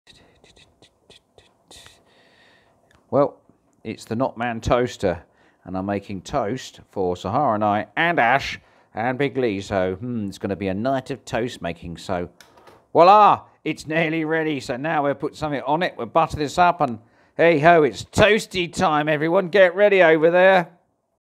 Hot Toast Time With The Knotman.
The knotman  is grilling  toast  ..for the green plant team..Team leader asher macCaddan MBE and team assistant Sahara knite and big lee Watkins...
and a lot of tea. nice.
The green plant team girls lady Charlotte grey and lady Mary charteris and lottie moss DJ jemma bolt and DJ Sarah story Keith brown Jason gooden Esquire and girlfriend alicia vikander and Phil  will meet up later to a talk about the environment mission.